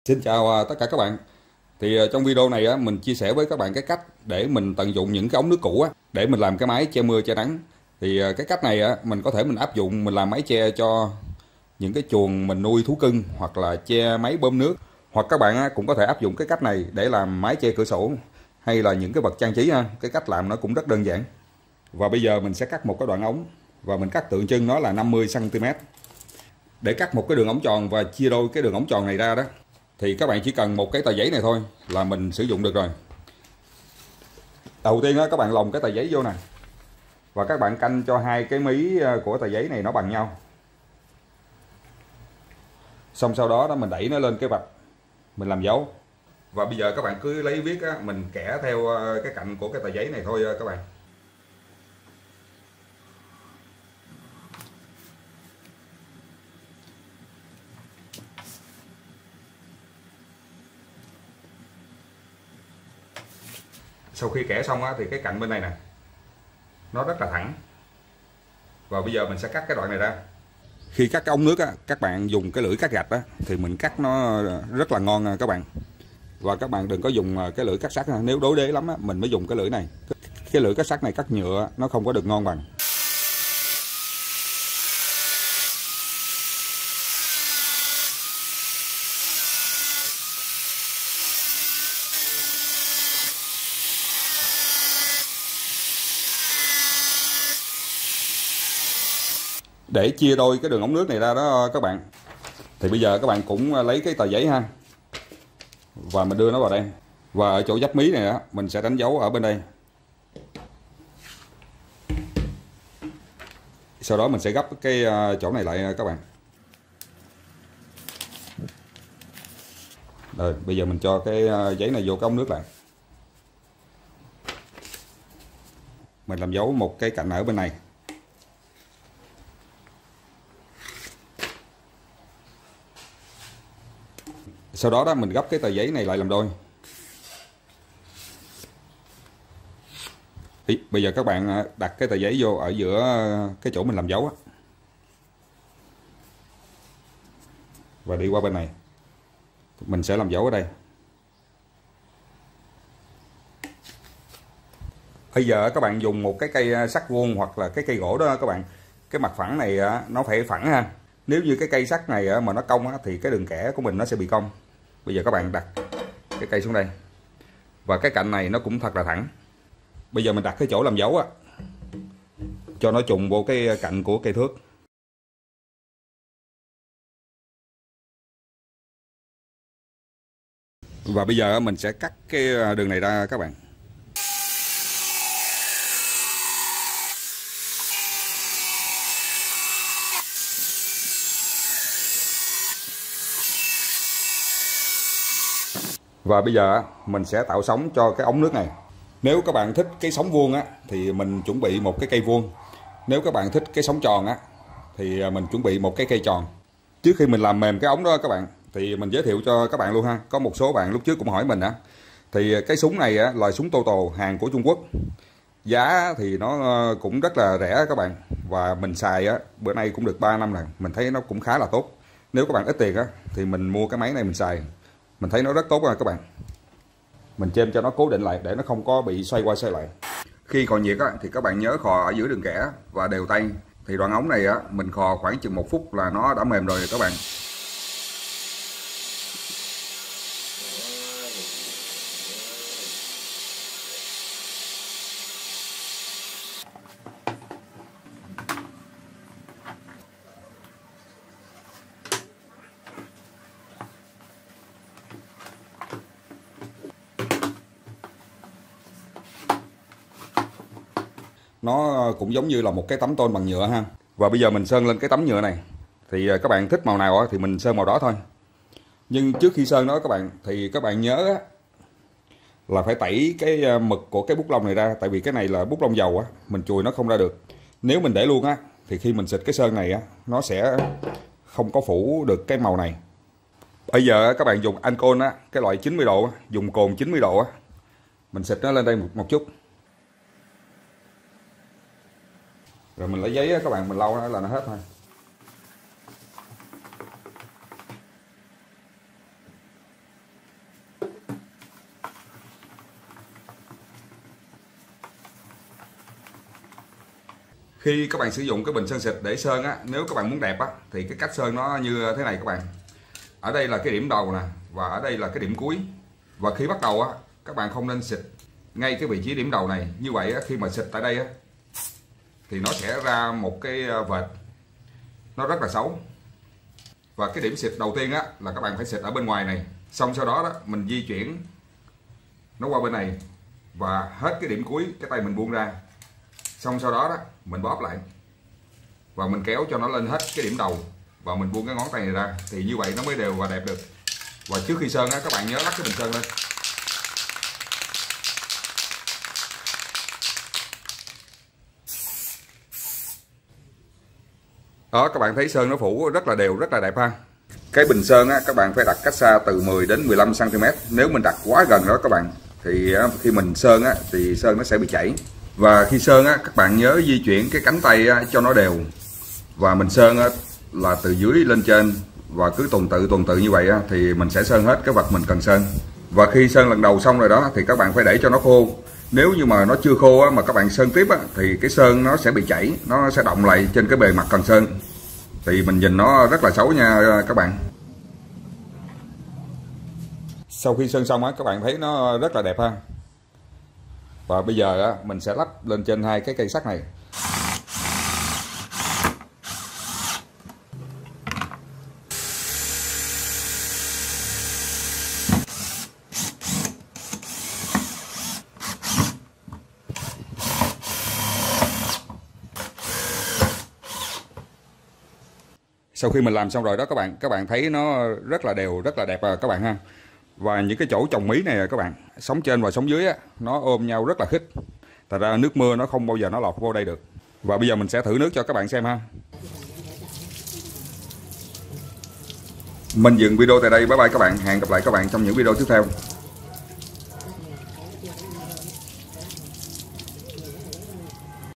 Mọi người sẽ thấy điều này tốt hơn nhiều với ống nước pvc _ Ý tưởng tuyệt vời
Chia sẻ với các bạn cách làm mái che mưa, che nắng bằng ống nước pvc, một ý tưởng tuyệt vời. Mọi người sẽ thấy điều này